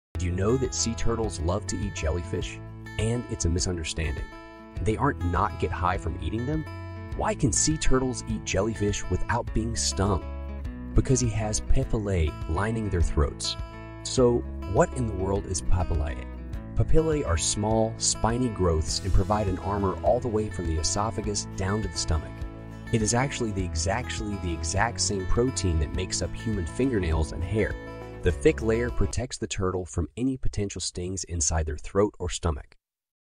why do sea turtles like to eat jellyfish
So, why do sea turtles like to eat jellyfish?

is one of the most unique looking animals. They look like the combination of pigs and elephants.